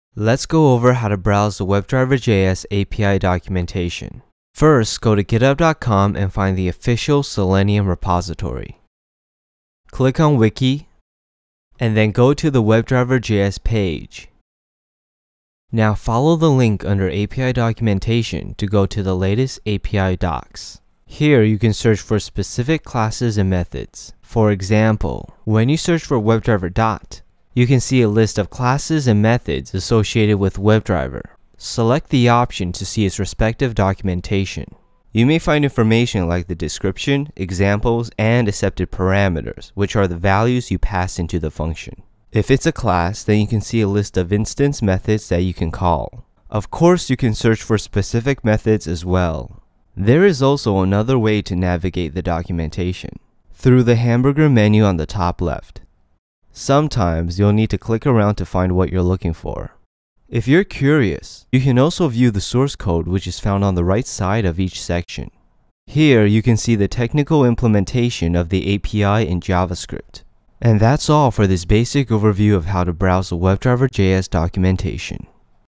Browsing the WebDriverJS API Documentation — Selenium WebDriver for Node.js
Learn how to browse and navigate the Selenium WebDriverJS API documentation.

In the docs, you can find information and examples about API classes and methods and be able to view the source code.